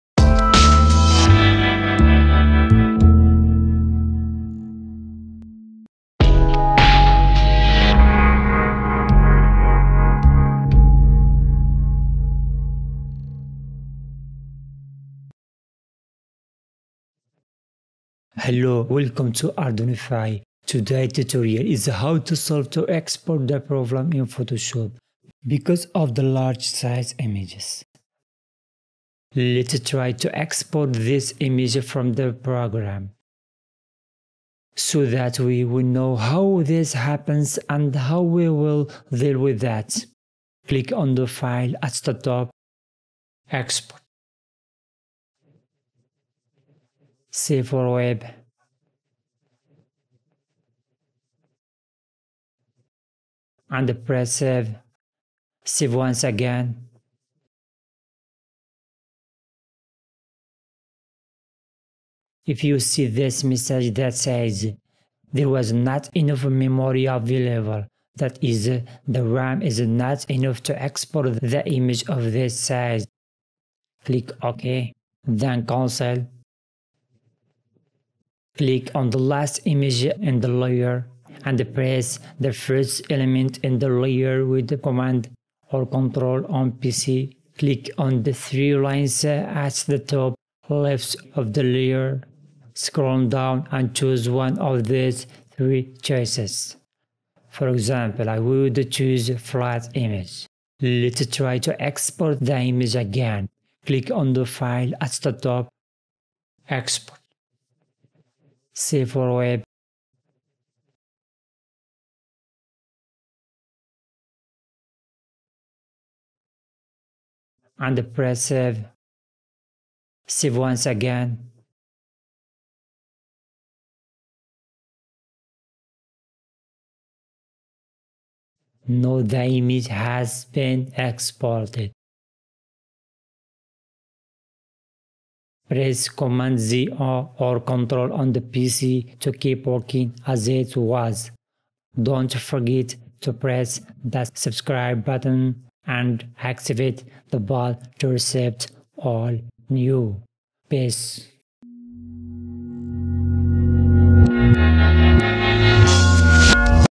How to solve the export problem in Photoshop Because of the large size images | Ardounify
Hello.
A lesson for Photoshop on how to solve the export problem because of large-sized images or because of insufficient RAM for exporting heavy-sized files